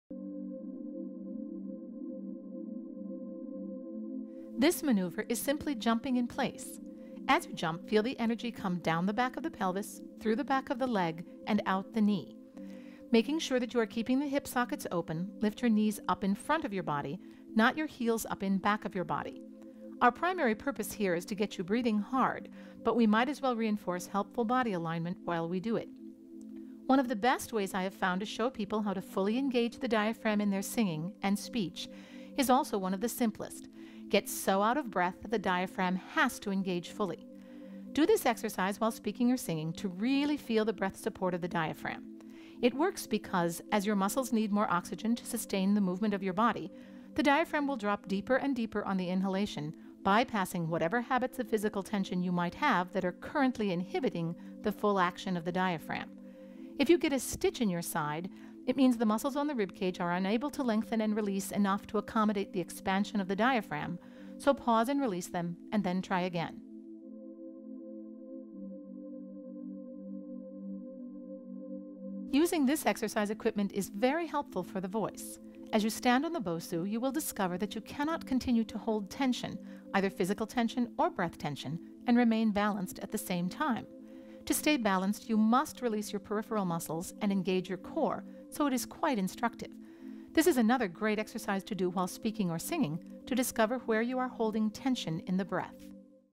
RELEASE TENSION FOR SINGING: Breath
Ruth Williams Hennessy guides you through exercises to release your breath in order to get the most out of your sound!